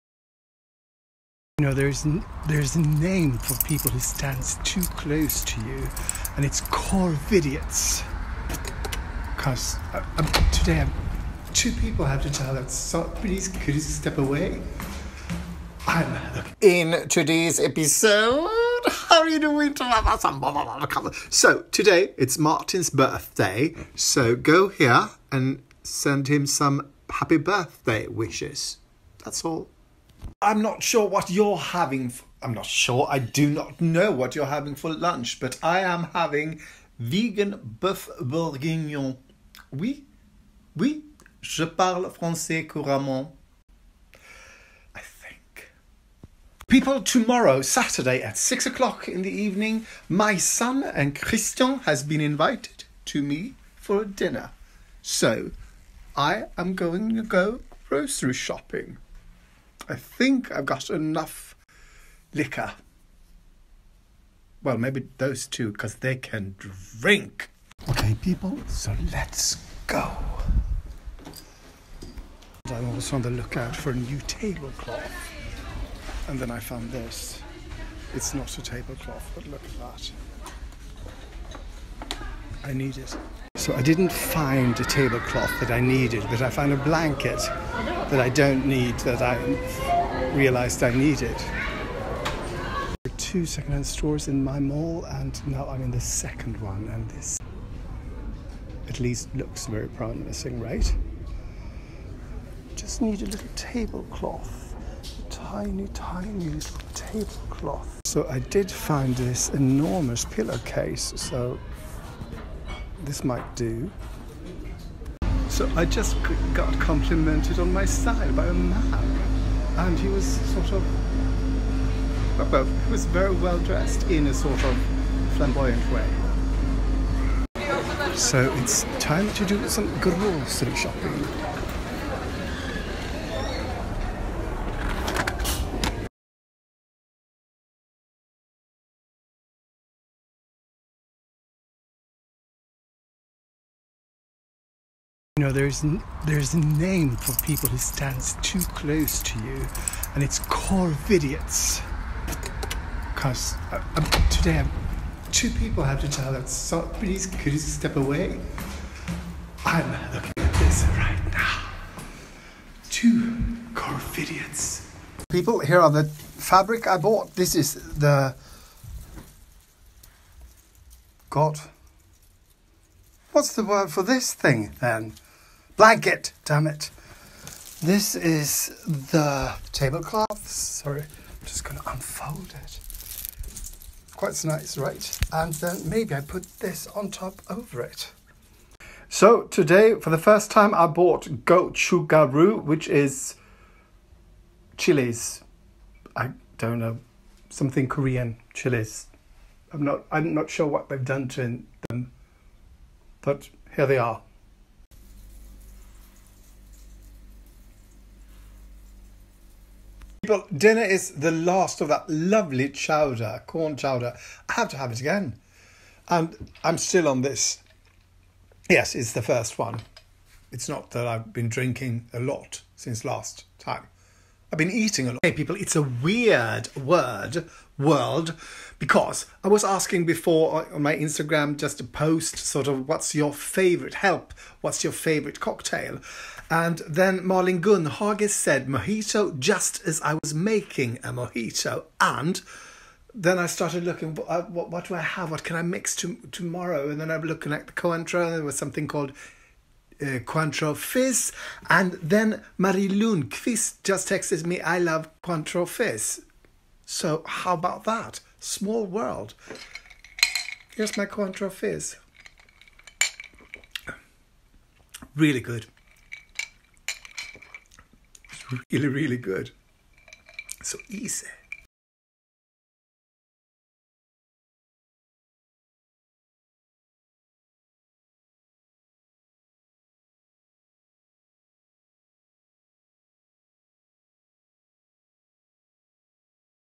201016 CORVIDIOTS, GOCHUGARU & TABLECLOTHS
In this episode I go SHOPPING for a new TABLECLOTH and GRICIERIES and come home with a new BLANKET, I get COMPLIMENTS for my STYLE by a man, and I meet some CORVIDIOTS.